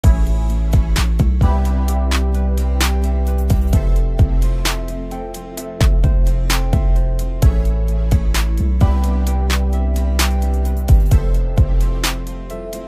Bronte Chesterfield Sofa sofa trendingshorts #
Introducing the epitome of classic elegance and timeless charm - the Bronte Chesterfield Sofa! ✨ Create a living room that's both inviting and stylish with this stunning piece. Whether you're hosting a soirée or simply enjoying a cozy night in, the Bronte is the perfect centerpiece.